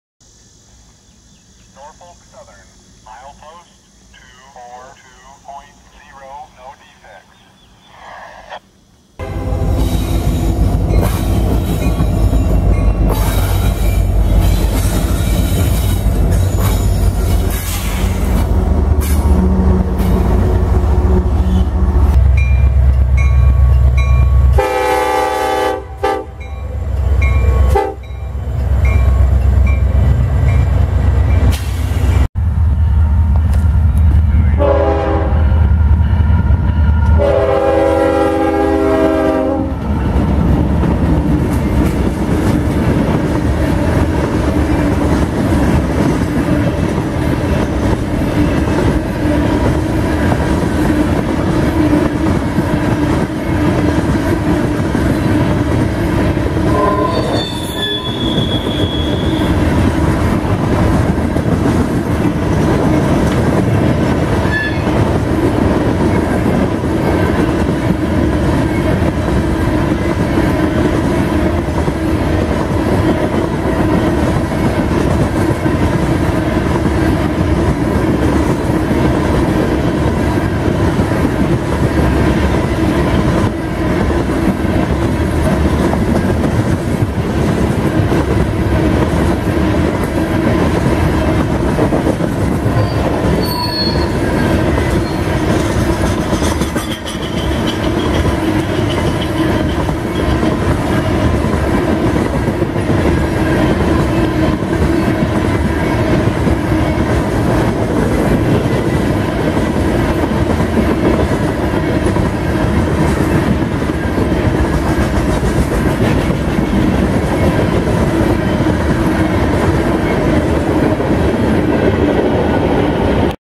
CSX N172 With Two UP Motors
Barely reached the crossing in time to catch this awesome coal train running at speed.